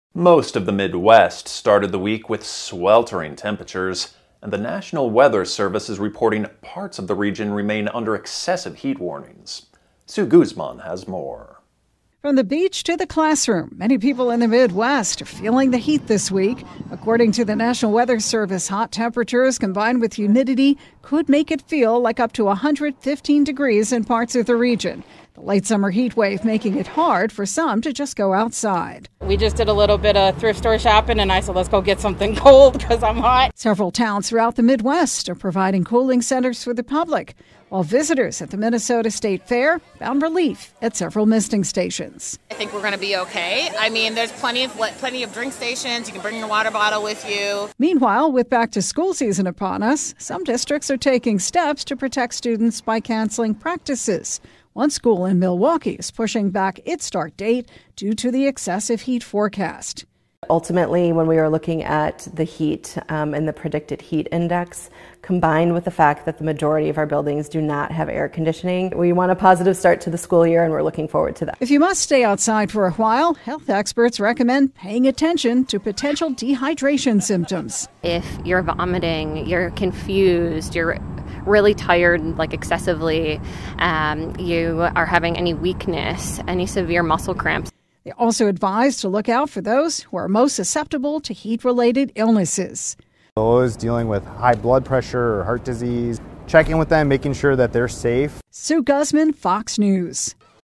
Midwest Faces Sweltering Heat, NWS Issues Excessive Warnings
Most of the Midwest started the week with sweltering temperatures and the National Weather Service is reporting parts of the region remain in excessive heat warnings.